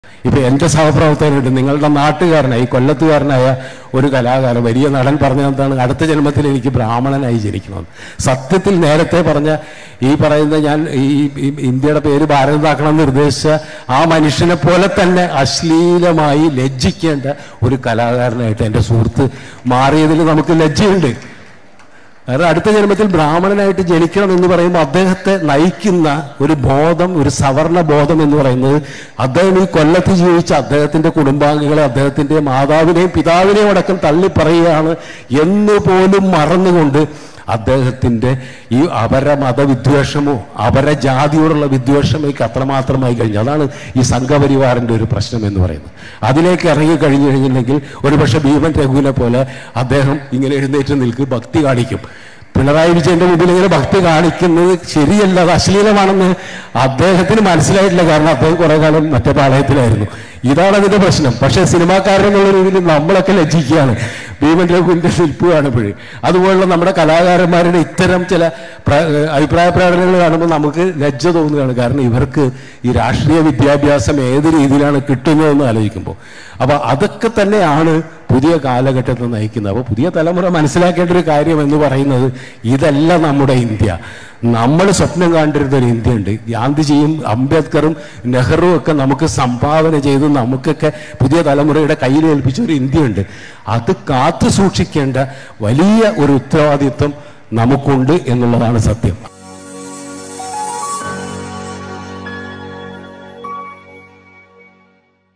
സുരേഷ് ഗോപി ലജ്ജിപ്പിക്കുന്ന കലാകാരനായി മാറിയെന്ന് സംവിധായകൻ കമൽ | Madhyamam
സുരേഷ് ഗോപി ലജ്ജിപ്പിക്കുന്ന കലാകാരനായി മാറി; സംഘപരിവാറിലേക്ക് ഇറങ്ങിയാലുള്ള കുഴപ്പമിതാണ്'; തുറന്നടിച്ച് സംവിധായകൻ കമല്‍